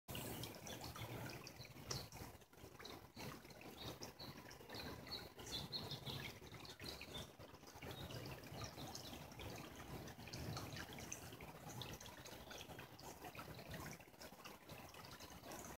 Fifteen Seconds of Country
Melt off from the recent snow storm is keeping this creek running.